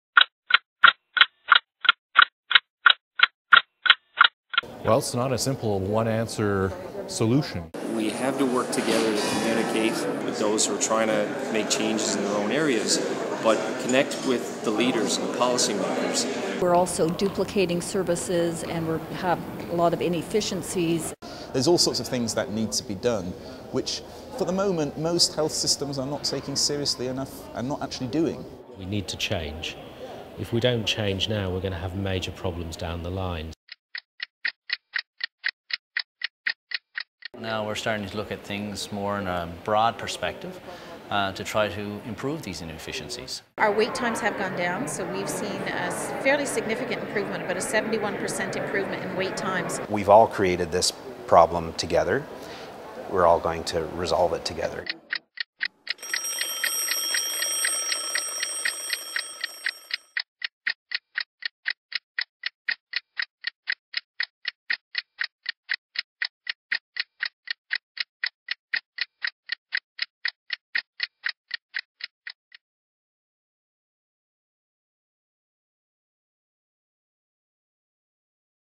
Why the long waits in Canadian health care?
A promotional video for the Health Council's upcoming video series on wait times. For more information on wait times in Canada download Progress Report 2013: Health care renewal in Canada and the Health Council of Canada portal